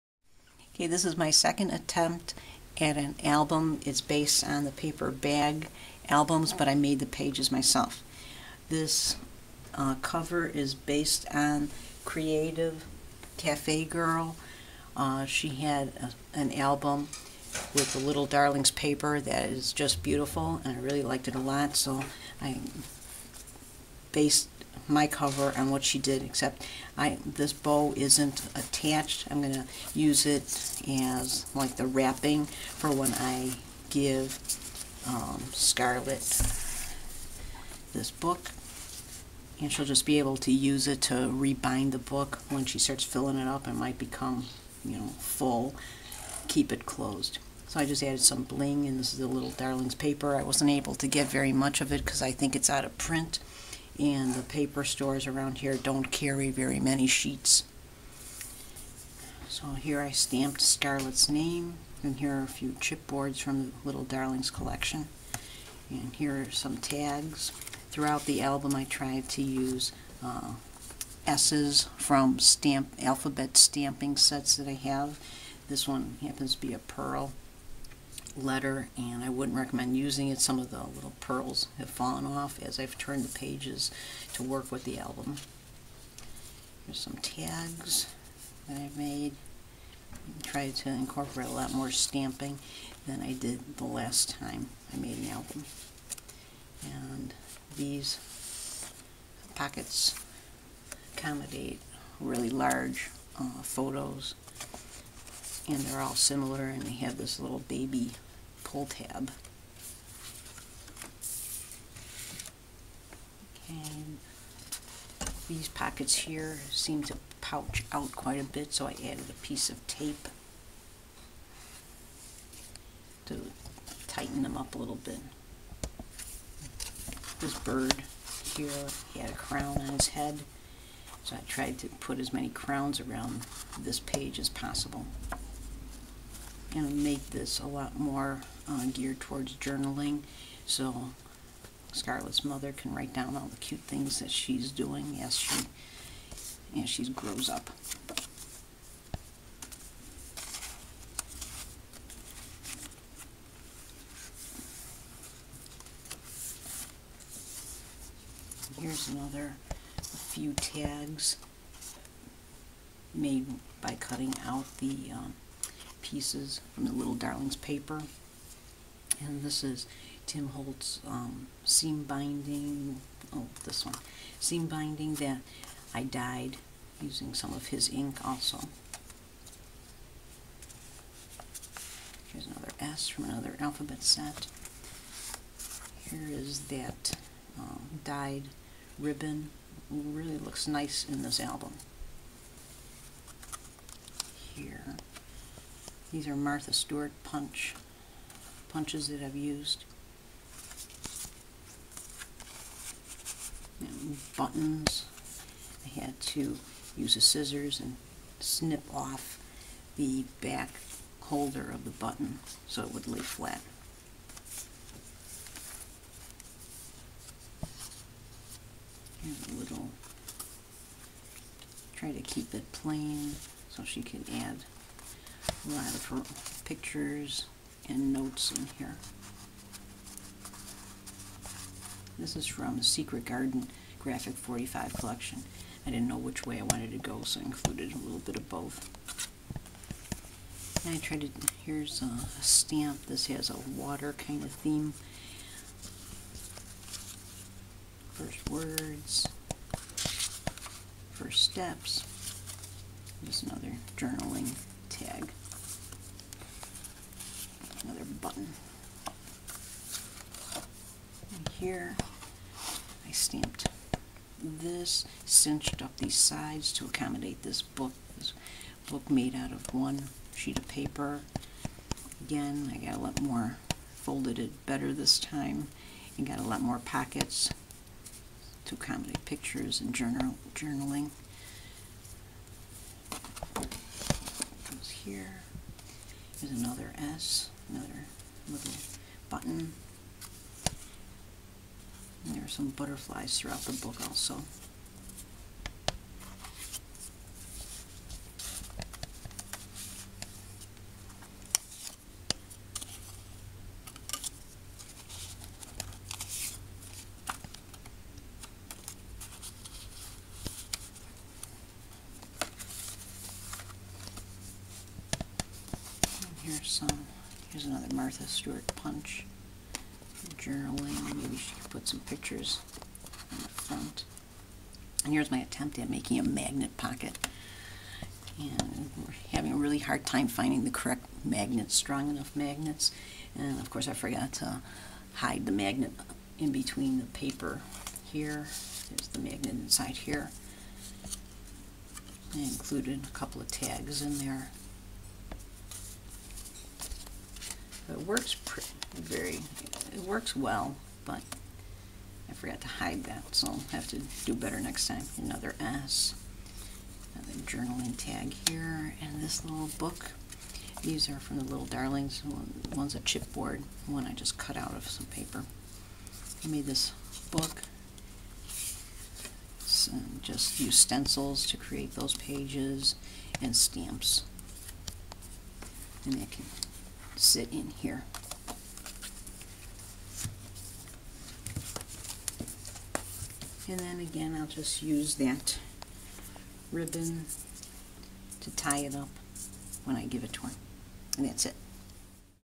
Mini Album 02 - Baby Girl
This is a baby album I created based on the paper bag construction.  I created my own pages using card stock in a variety of shades of pink.

I used creativecafegirl as my model for the cover.  Her baby girl and baby boy albums are both so beautiful.